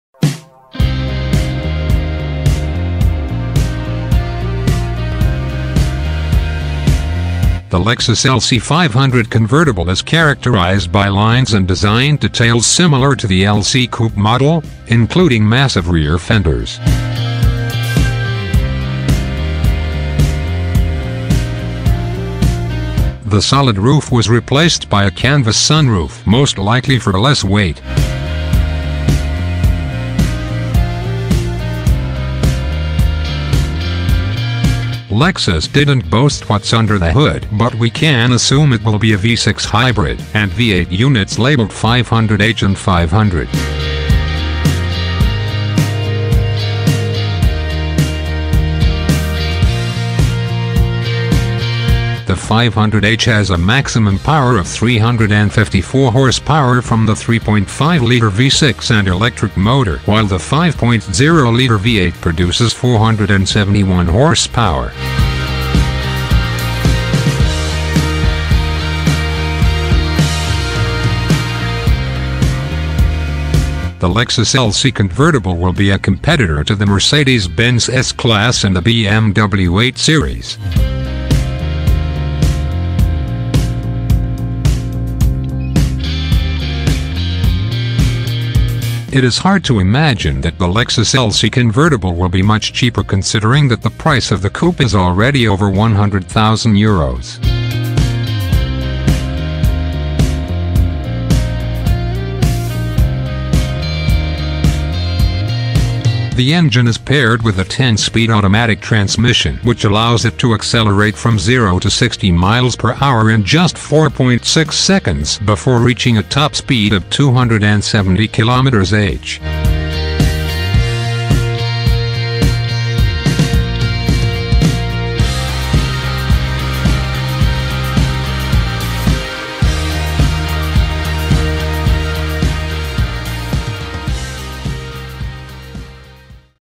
2021 Lexus LC 500 Convertible - all details, picture and video parts
The soft-top convertible model is new for 2021.
Cabin design and interior materials are extraordinary.
Effective at being both sporty and comfortable.
The convertible top doesn't dampen the LC's quiet refinement.